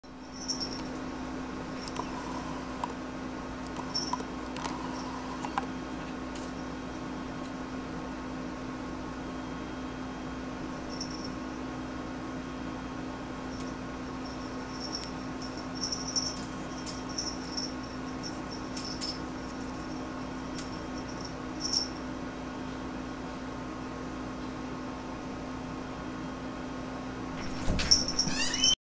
Tough Kitten Takes A Stand...
This kitten is SO CUTE as she takes on her big step-brother!  She looks like a little Koala bear:)  They were just playing, but she looks instense!  Oz, the big black cat, is featured in some of my other videos.